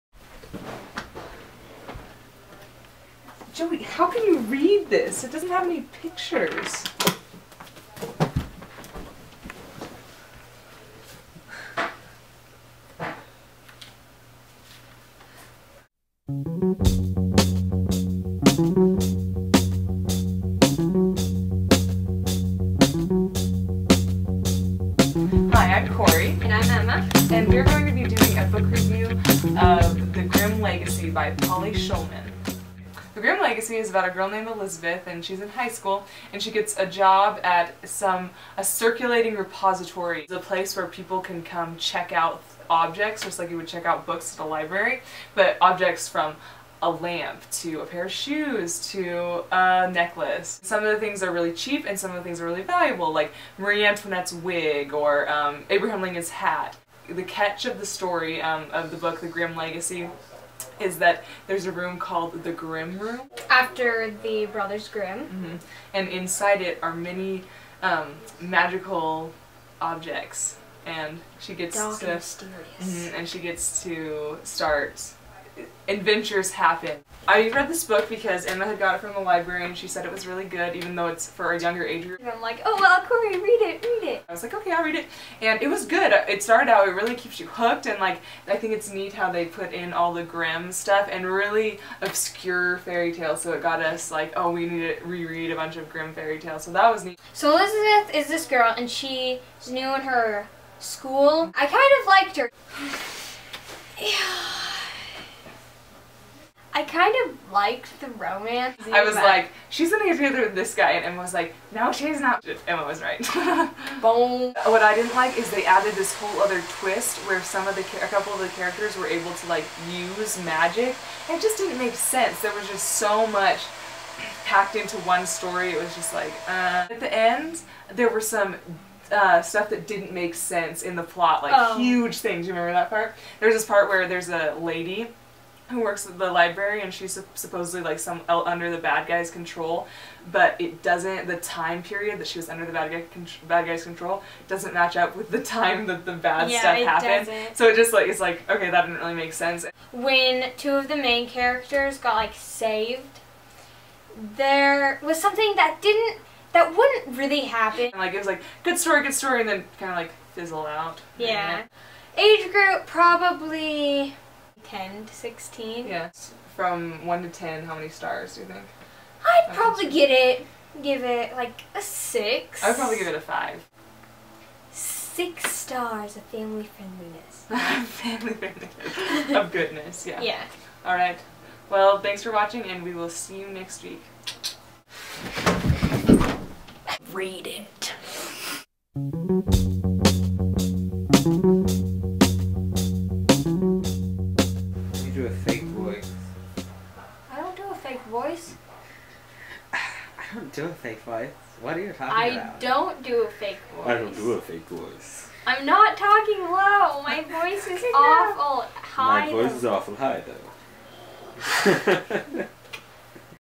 READ IT the Grimm Legacy
Ladies and Gentlemen, I introduce to you: my sister, the youngest member of our household, the ravishing, the irreplaceable: Emma Hehn!

Here is her and me giving a book review of The Grimm Legacy by Polly Shulman.

P.S. That voice talking to Emma at the end of the video is our brother Jacob. He's refusing to do a book review for you guys even though he LOVES to read.